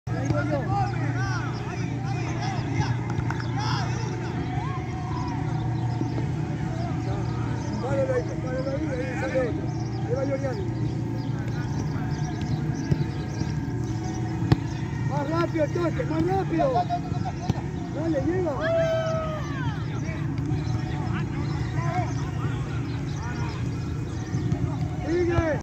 Trabajo variados: 1)pases y remates al arco 2) 1vs 1 3)tiro penal, 4) tiro libre directo 2023.(3)
Gracias a Dios por un día más de vida, salud y permitirnos trabajar, aquí en la cancha sintética de La Pista (Cisne 2) preparándonos para el torneo de segunda categoría.

Práctica de penal, tiro libre directo, 1 vs 1, pases y remates al arco.

Jueves 18/05/2023.